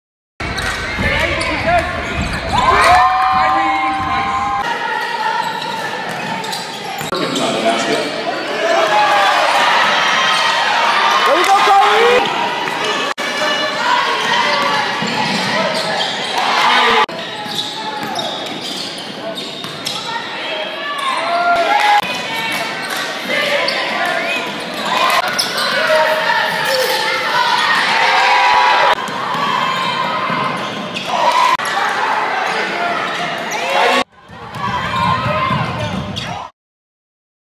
Kairee price 17 points vs Antioch
Jr combo guard averaging around 15-17 points right now. glencliff highschool Nashville tn